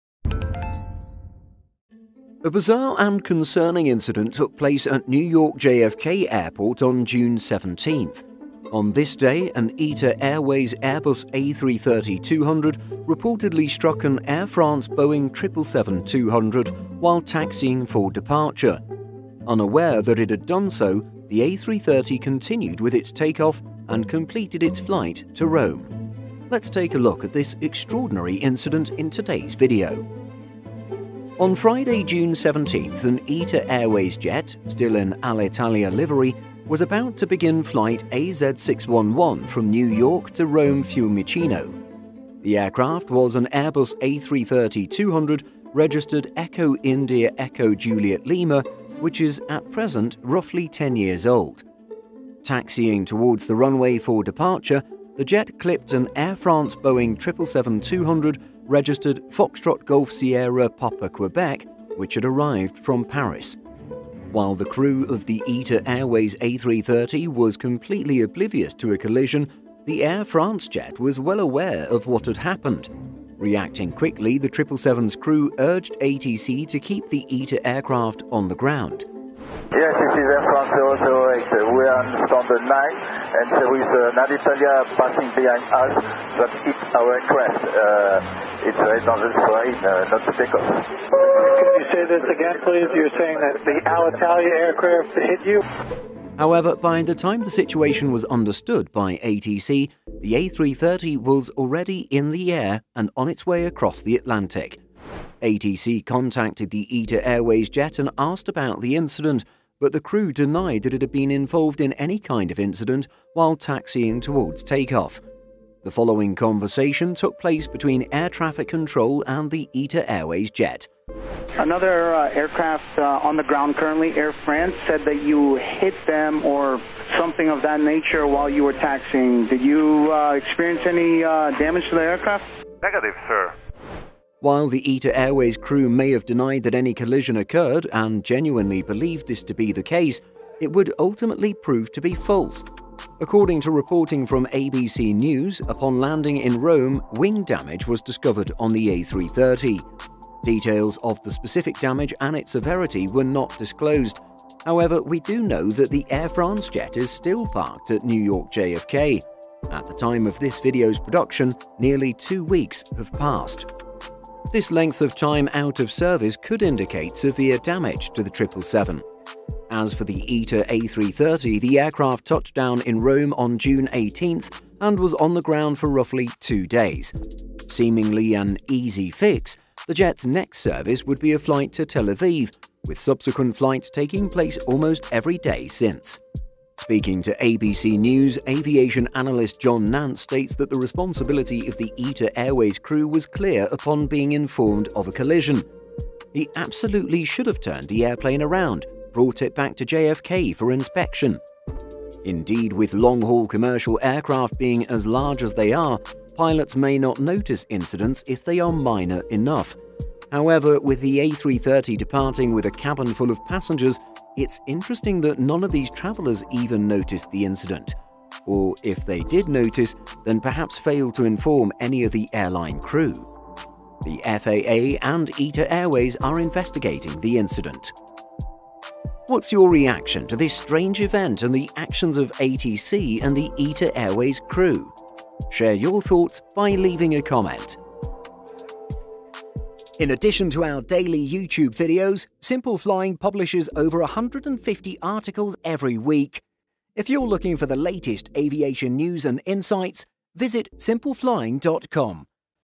ITA Airways Airbus A330 Hits Air France Boeing 777 And Takes Off
A bizarre and concerning incident took place at New York JFK Airport on June 17th. On this day, an ITA Airways Airbus A330-200 reportedly struck an Air France Boeing 777-200 while taxiing for departure. Unaware that it had done so, the A330 continued with its takeoff and completed its flight to Rome. Let’s take a look at this extraordinary incident in today’s video…